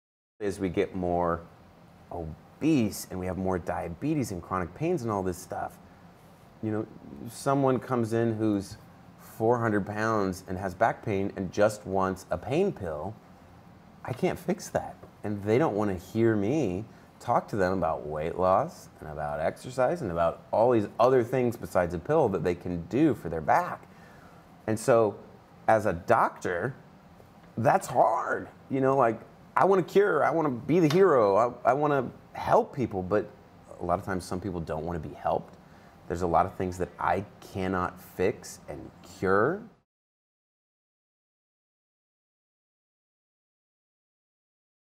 IN THE E.R.: Sometimes Patients Don't Want Real Help
IN THE E.R.: In the US, we are getting more obese, experiencing more chronic illness, seeing more people with diabetes. Many times though, these patients who have something wrong with them, the best way to cure their pain is usually by losing weight which is exercising and eating right. But they may only want a pill.

As a doctor, this is hard. I can't fix this or change them. They don't want to hear me or talk to me about anything other than receiving what they already have their mind decided on. I want to help them, but a lot of times I get patients who don't want to be helped. This is a conscious choice and it's hard to watch someone leave the hospital that I know can get better in a healthier and longer lasting way.

Dr. Larry Burchett, ER Doctor and Author of "The Gentleman's Diet"